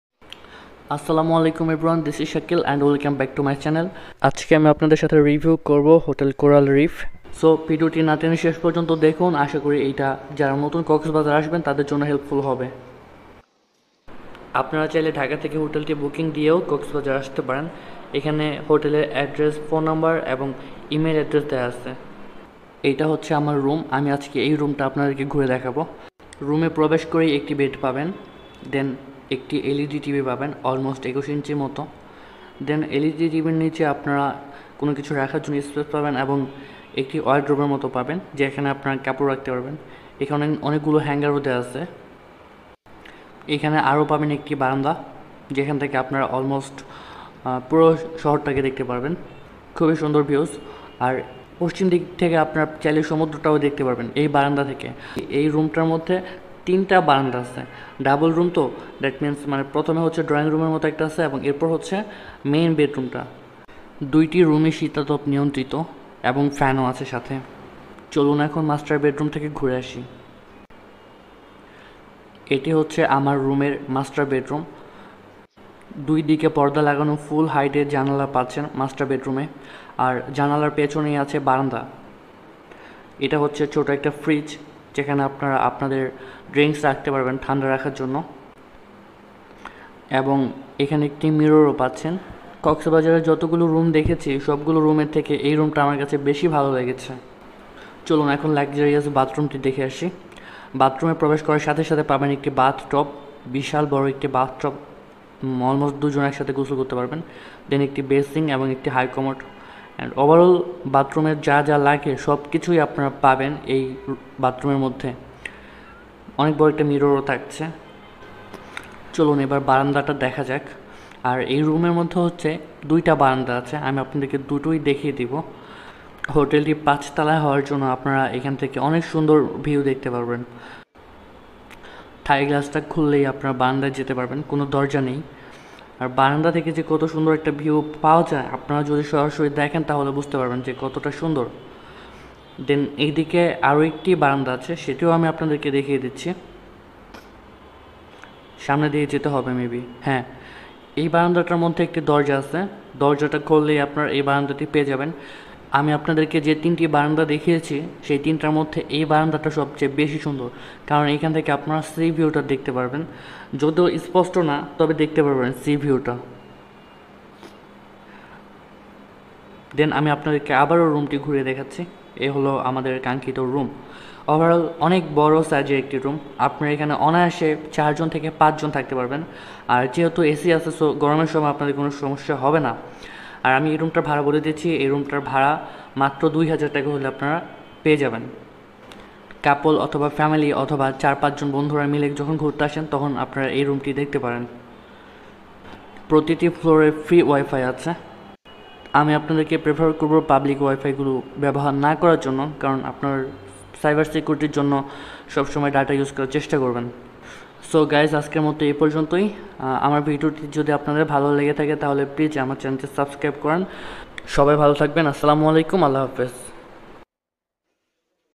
Hotel Coral Reef | Sea View Honeymoon Sweet Room Price In Cox’s Bazar 2022
This is hotel Coral Reef's luxurious couple/family A/C room that you can book for around 2000tk in off season. Price will be higher 500/700 in season (November, December, January). The room is so clean and well furnished with premium quality room and bathroom fittings. I have a good experience here. I highly recommend this hotel for specially newly married couple and 4 members' family.